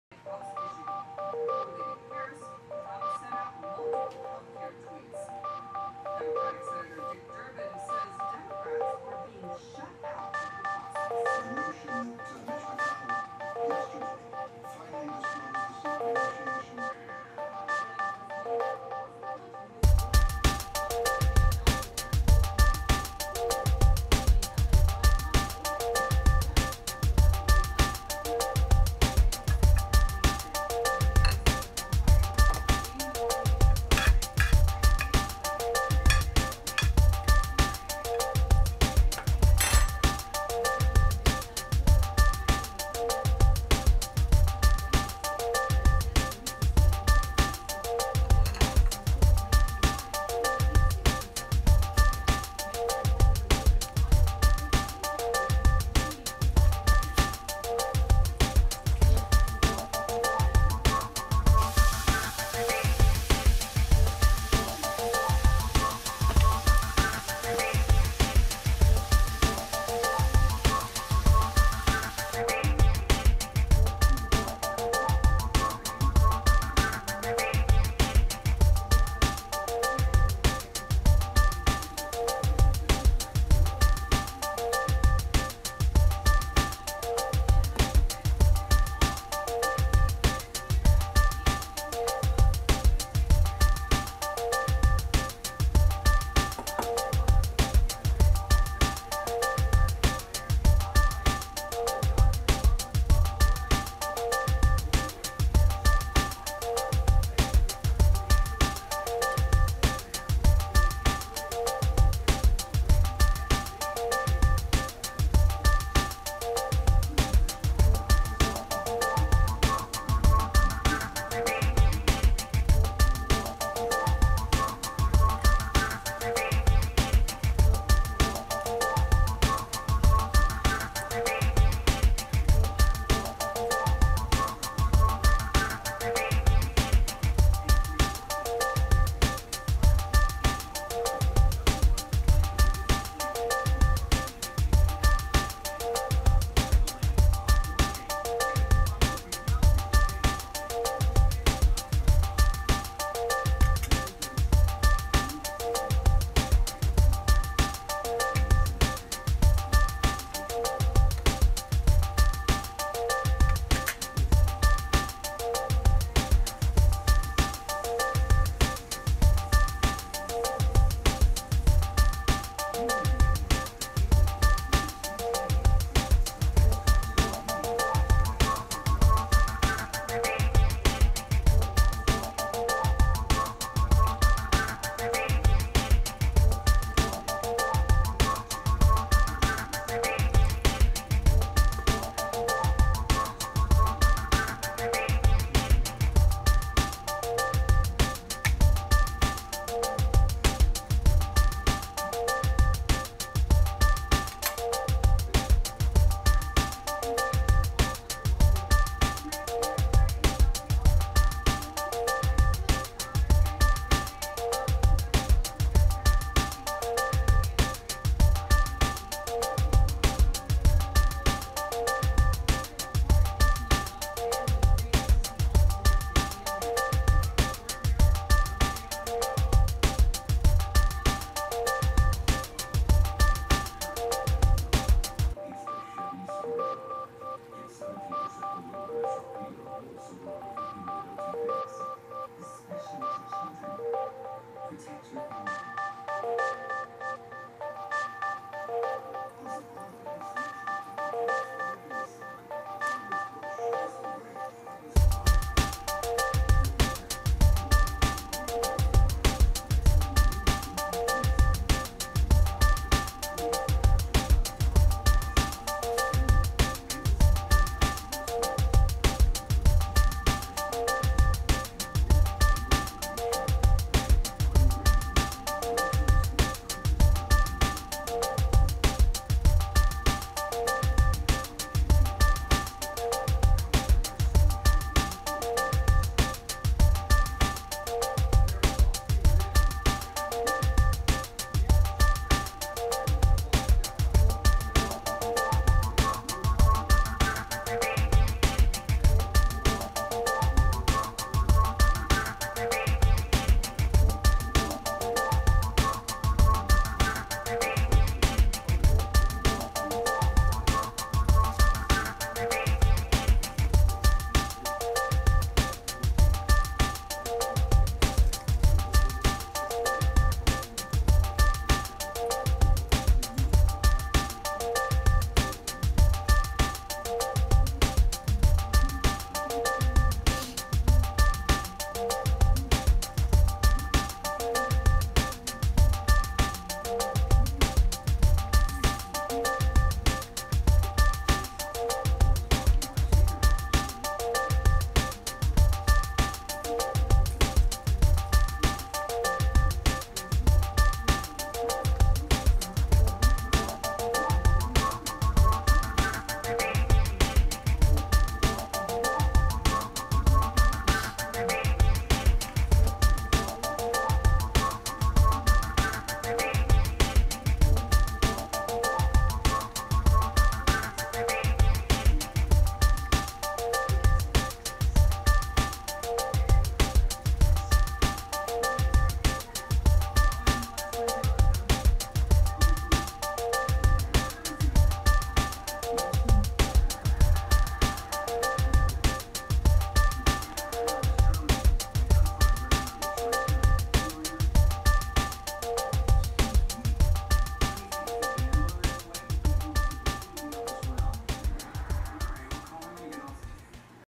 How to make Ground Beef and Potatoes Recipe
This is my simple recipe for ground beef with potatoes & veggies.

Ingredients:
potatoes
green unions
cabbage
carrots
green beans
salt
pepper
vinegar